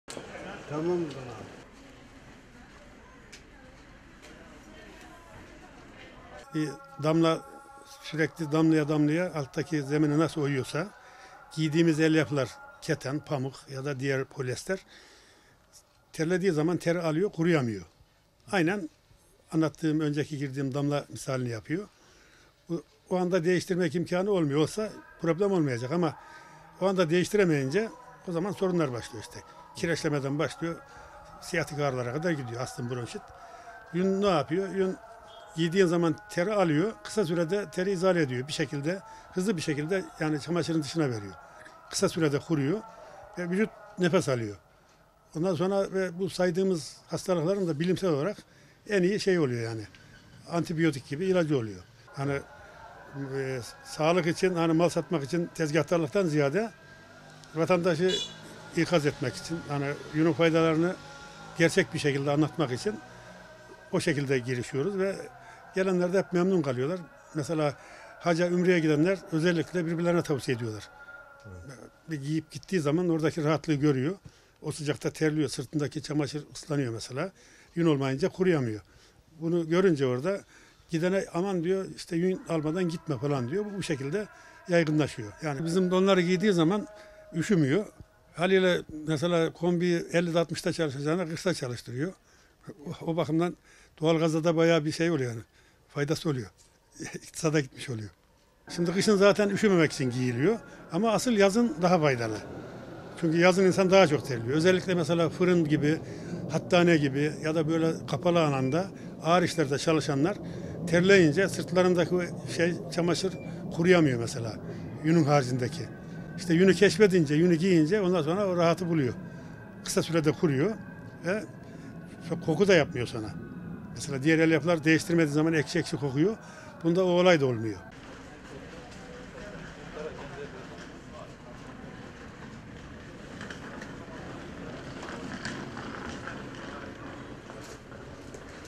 Bursalı Esnafın Bu Sloganı İlgi Çekiyor..
Havaların soğumasıyla birlikte vatandaşlar soba ve kombilerinin derecelerini yükseltirken, bir iç çamaşırı mağazasında yazan ‘Bursa için don uyarısı’ ve ‘doğalgaz faturasını düşüren donlar burada’ afişi dikkat çekti.

Meteorolojinin önümüzdeki günlerde yurt genelinde kar yağışı beklendiğini duyurması yün iç çamaşırı satışlarını artırdı. Bursa'da yıllardır yün iç çamaşırı satışı yapan Naci Bağcı, ilginç sloganlarıyla vatandaşların dikkatini çekmeyi başardı. Mağazanın dış camına ‘doğalgaz faturasını düşüren donlar burada’ afişi asan Bağcı, içeride bulunan rafa ise, ‘Bursa için don uyarısı’ döviziyle dikkat çekti. Bağcı, "Hava soğuklarının kendisini iyiden hissettirmesiyle vatandaşlar, soba ve kombilerin derecelerini yükseltmek yerine farklı ısınma yollarına başvurdu.
Kış kendini iyiden hissettirmeye başladı. Ancak yün içlikleri genelde üşüyenler, çok terleyenler ve sıyatik ağrıları olanlar tercih ediyor. Yünde bulunan boru şeklindeki lifler vücuttaki teri dışarı atarak insanı hasta olmaktan koruyor. Yün çok sağlıklı bir ısınma aracı. Yün giyenler kendini soğuktan sağlıklı bir şekilde koruyor. Bir de yün diye satılan orlonlar var, her yün diye satılan içlik yün değildir. Vatandaşlar yün içlikleri iyi bildikleri yerlerden alsın. Kıyıda köşede 5-10 liraya satılan ürünler yün değildir. Vatandaşlar aldanmasın. Hakiki yün sağlıktır. Biz yazın bile müşterilerimize yün içlik satıyoruz. Çünkü yün teri emmiyor” dedi.
Yün olmayan elyafların teri alınca ıslandığını ifade eden Bağcı, “Pamuk ise geç kuruyor. Aynı zamanda koku da yapıyor. Ama yünde böyle bir durum yok. Yün kolay kolay teri emmediği için hasta yapmıyor. Hekimler astım ve bronşit gibi hastalıkların çaresinin yün olduğunu söylüyor. Biz de bundan yola çıkarak müşterilerimize yünü tavsiye ediyoruz. Yün, elyaf gibi teri alıyor, ama teri vücutta ıslak mendil gibi tutmuyor. Kısa süre tutup sonra dışarı transfer ediyor. Böylelikle hem vücut rahat ediyor, hem de vücuttaki ağrıları alıyor. Dolayısıyla vatandaş yünden sapmıyor. Bu yünleri giyen vatandaşlar vücutları ısındığı için doğalgaz vanalarını kısmak zorunda kalıyor, hal böyle olunca doğalgaz faturaları daha az geliyor. Hem sıhhat, hem ekonomik bakımdan insanlar kârlı çıkıyor. Giydiğimiz elyaf, keten, pamuk veya polyester de terlediğimiz zaman teri alıp kuruyamıyor. O anda değiştirme imkanı olmayınca hastalıklar baş gösteriyor. Yün ise teri alıp kısa sürede kuruyor. Vücut nefes alıyor. Bir çok hastalığa da iyi geliyor. Vatandaşı ikaz etmek için böyle sloganlarla sesleniyoruz. Bizim donları giydiği zaman vatandaşlar üşümüyor. Haliyle kombiyi veya sobayı 50-60’ta çalıştıracağına 40’ta çalıştırıyor. Doğalgaza da faydası oluyor" diye konuştu.